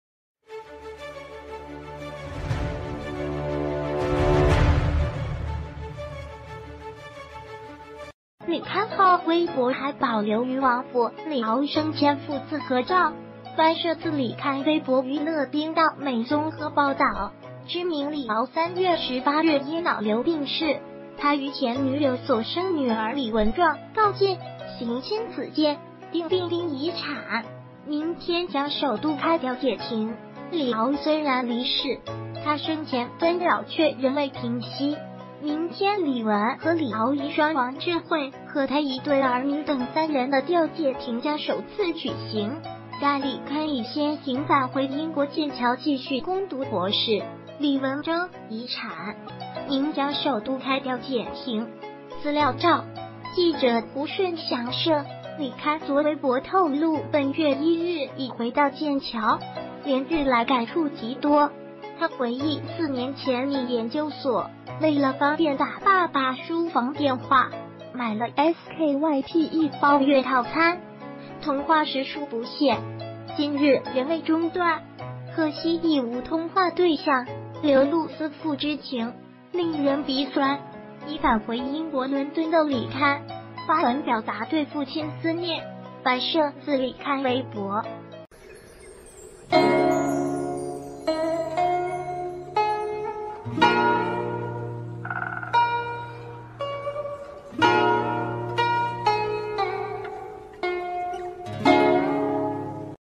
李文爭遺產明首開調解庭 李戡返劍橋攻讀博士
李戡（後）微博還保留...